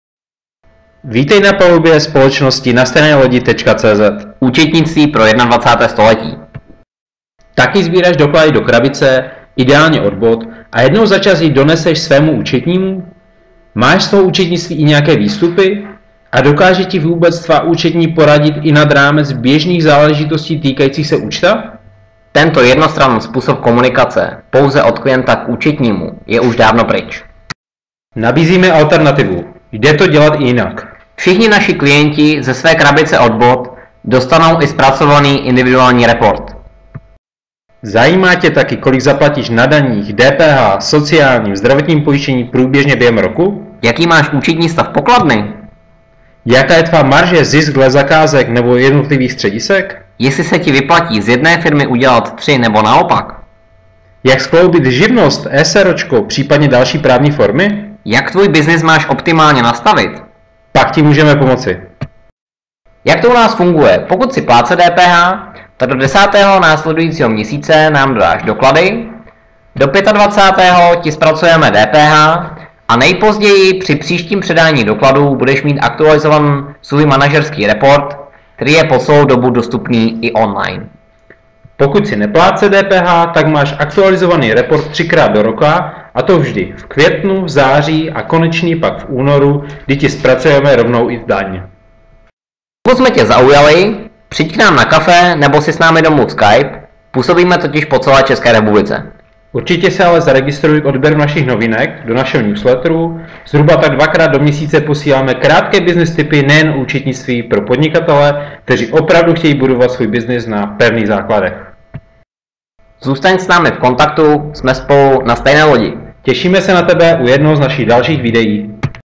Nastejnelodi cz - jak fungujeme a kdo je náš zákazník
Účetnictví s přidanou hodnotou. Vytváříme pravidelné manažerské reporty pro naše klienty. Na účetnictví se díváme očima podnikatele.